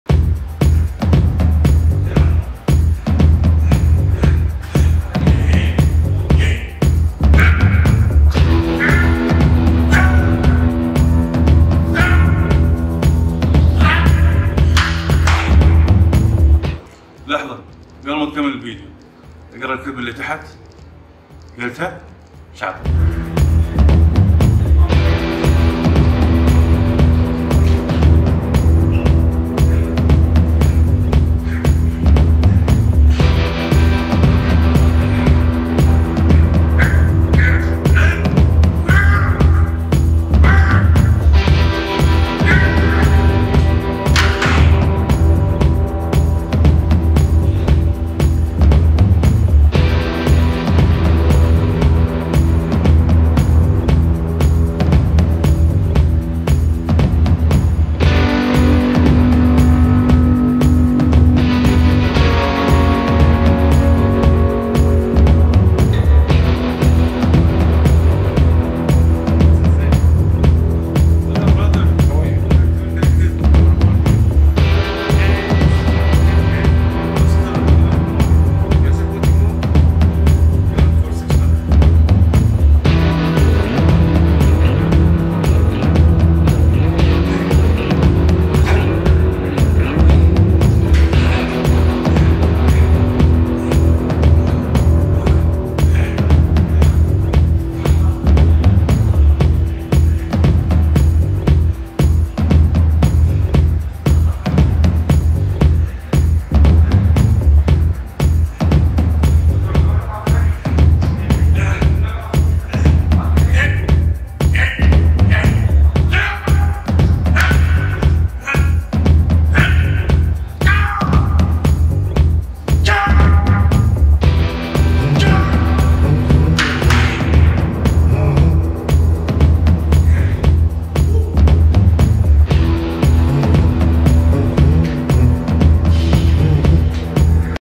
my new recored 601 kg leg press
my first time doing a 601 kg leg press that am proud of
my last record was 500 kg so I=am so glad that I could DO IT